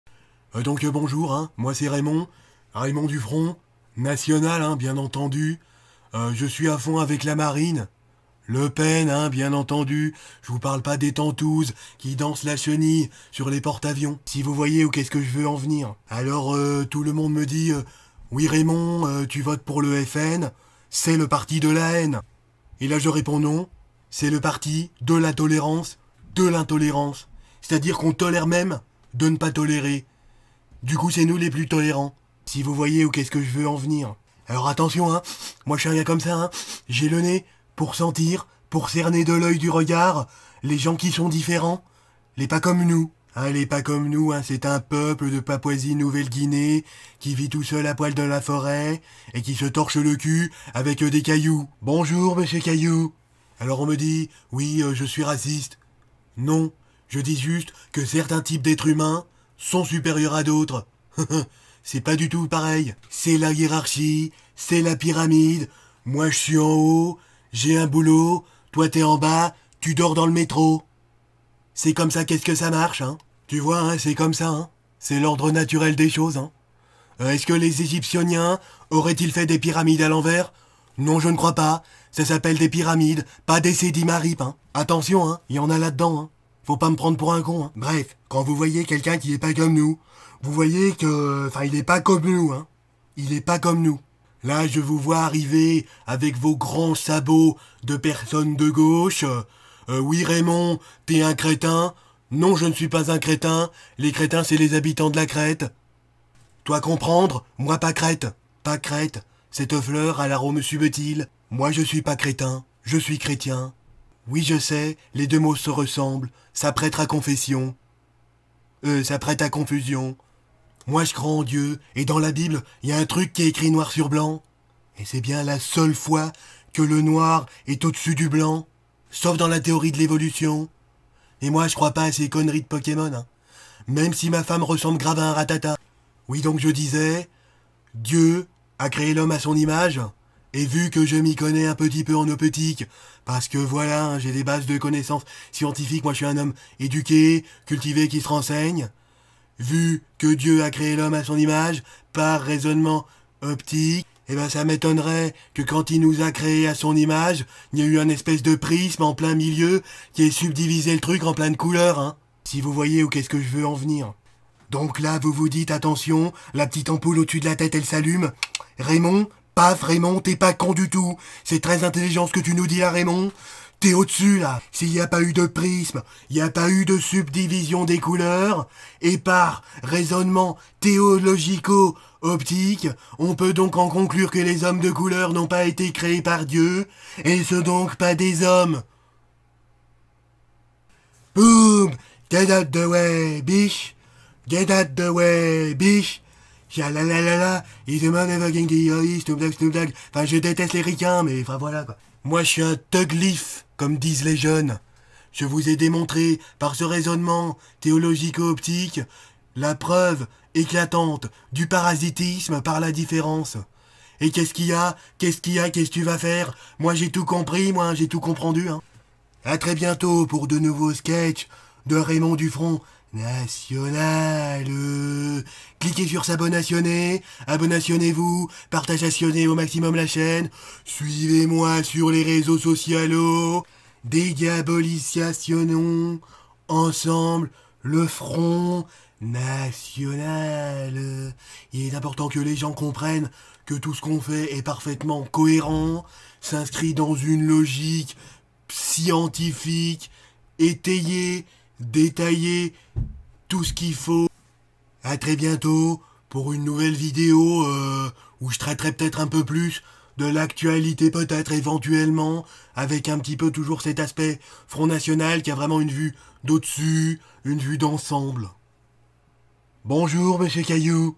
RAYMOND DU FRONT (NATIONAL, BIEN ENTENDU) - Superflame
J'ai créé le personnage de Raymond du Front,
parce-qu’on en connait tous des gars comme lui !

Bien pétri de certitude et persuadé de voir ce que les autres ne voient pas !

Raymond est inspiré d'un gars que j'ai croisé, je sais pas encore si je ferai d'autres épisodes, j'ai écrit celui-ci d'une traite :p